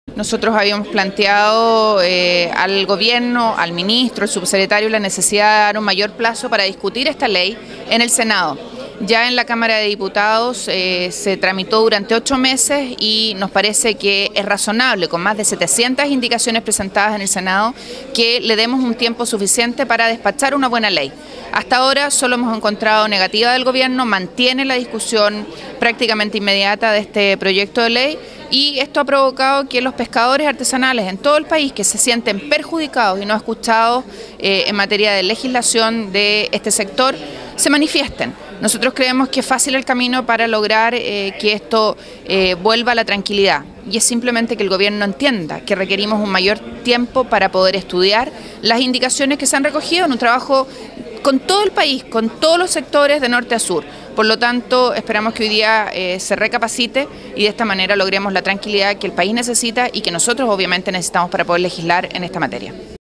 Mayor tiempo para legislar el proyecto Ley de Pesca pide al Gobierno Senadora Rincón
Frente a la huelga de los pescadores artesanales, la Senadora Rincón solicitó al Gobierno, más tiempo para legislar el proyecto de Ley de Pesca que es rechazado por los pescadores de todo el país.